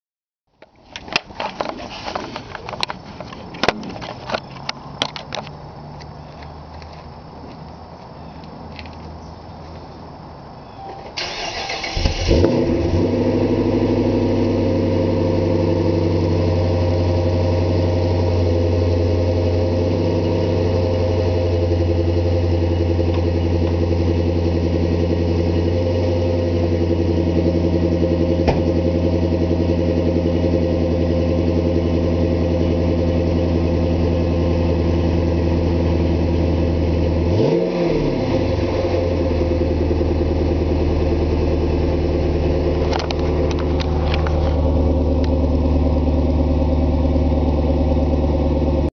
2011 Dodge Charger R/T Fall Startup and Rev
Just another startup and rev of my 2011 Charger R/T with a Flowmaster Force II cat. back.